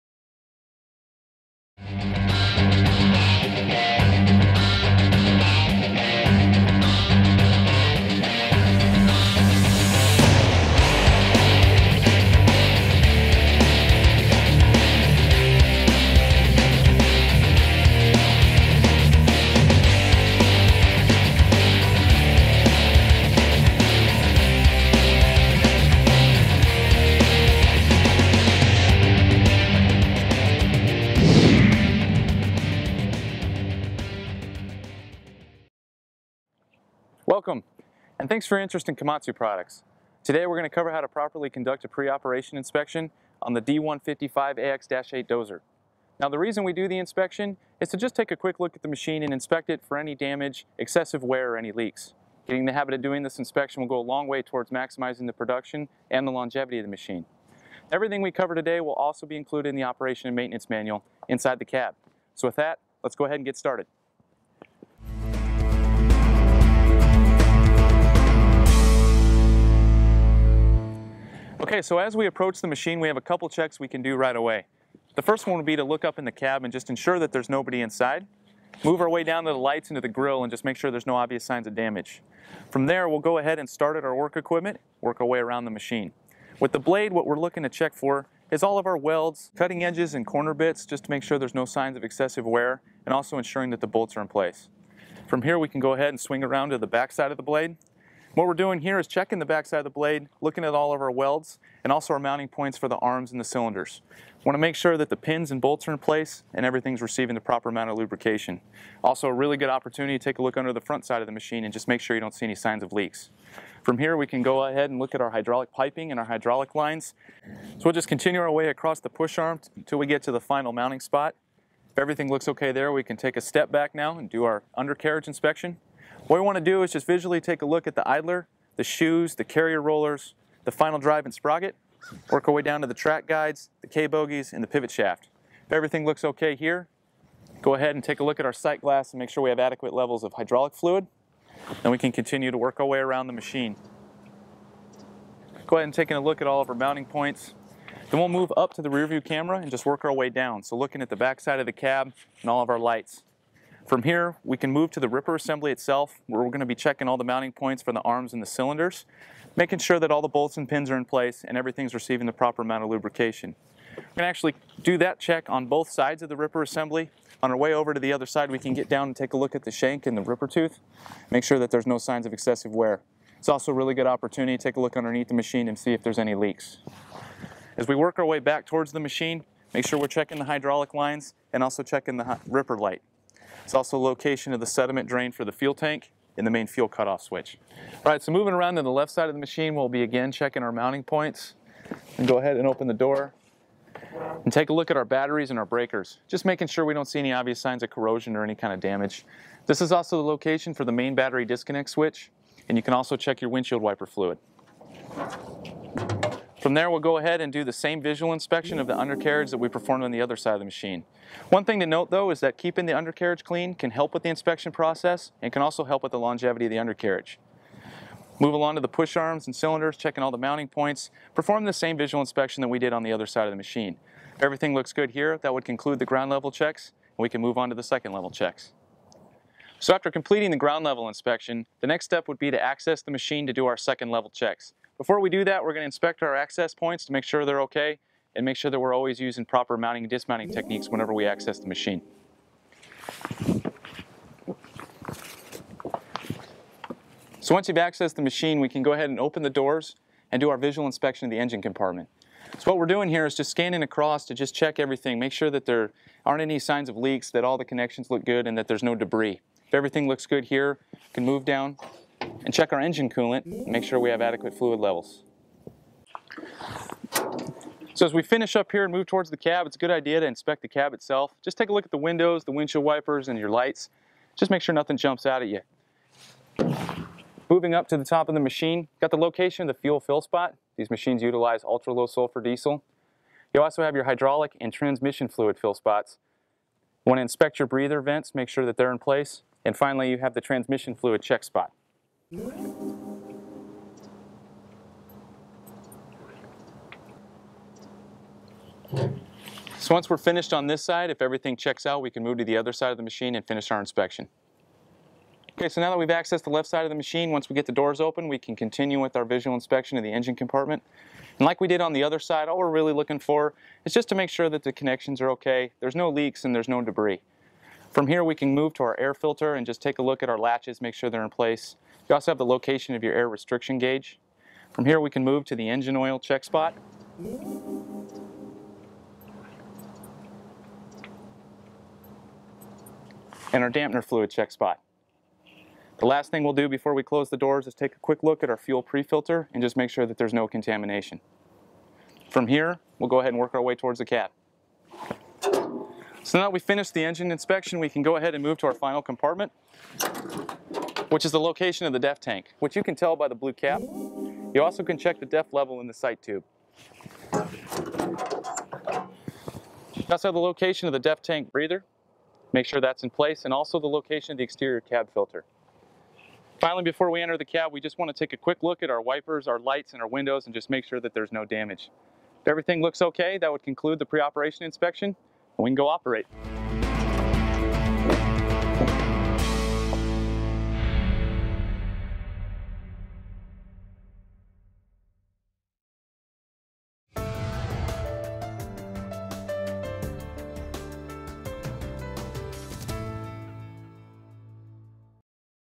Komatsu D155AX-8 dozer - pre-operation inspection
The new Tier 4 Final D155AX-8 dozer incorporates well-proven Komatsu engine emissions technology that does not interfere with daily operation.

Class-leading engine horsepower, large capacity Sigmadozer blade and automatic lockup torque converter efficiently move massive amounts of material while using less fuel than prior model. The operator cab is loaded with features to help maintain operator comfort during the work day.